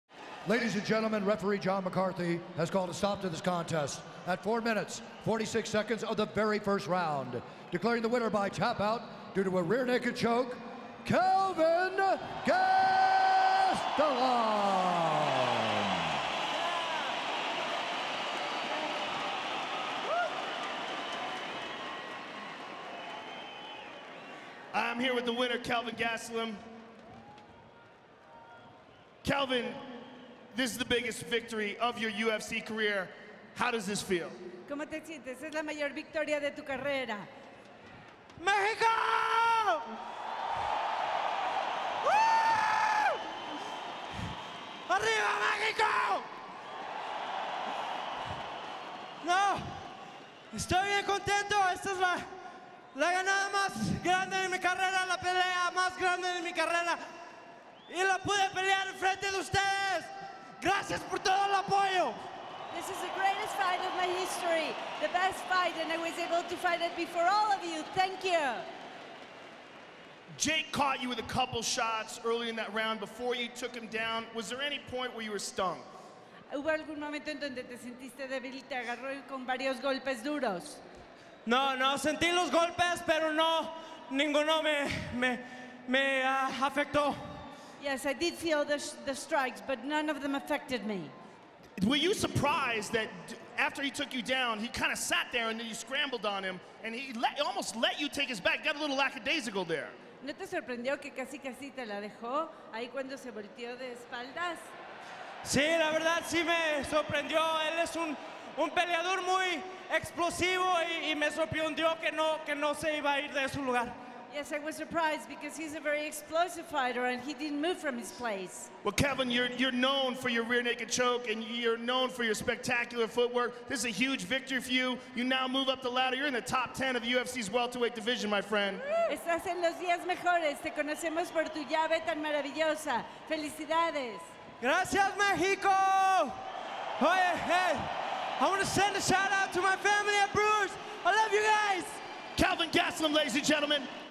UFC 180: Kelvin Gastelum Highlights
Kelvin Gastelum speaks to UFC color commentator Joe Rogan immediately following his big first-round submission win against UFC veteran Jake Ellenberger at UFC 180.